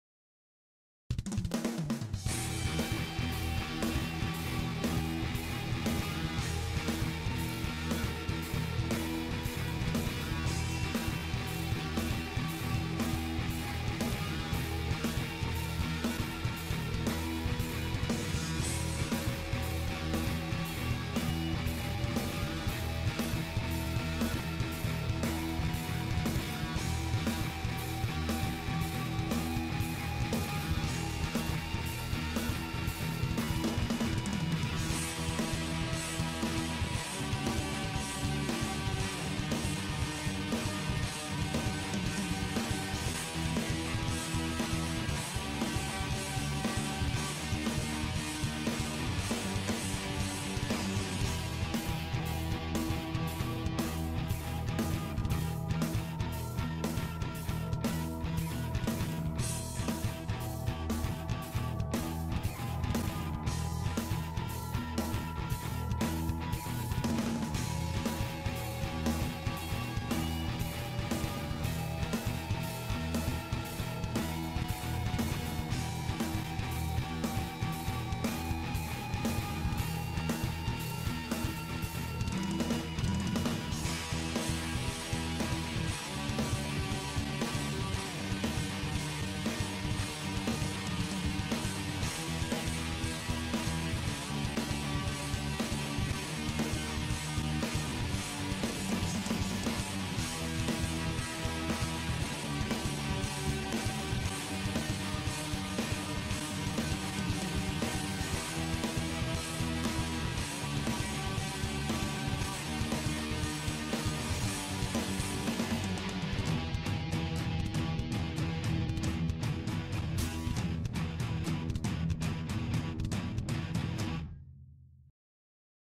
Fujifilm Finepix HS10 Slow Motion
Testing HS10 video slow motion capabilities. Frame rates in this video range from 60 fps to 1000 fps. Some 60 fps scenes have been doctored with Twixtor. Enjoy!